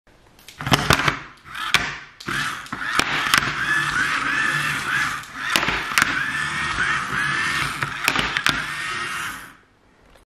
Hammerhead Shark - Hammer Bot - Playable Battle Robot Kit
Playable Battle Robot Kits showcase - Hammerhead Shark - Hammer bot!
Build it and drive it, all the parts you need including tools and batteries included in the kit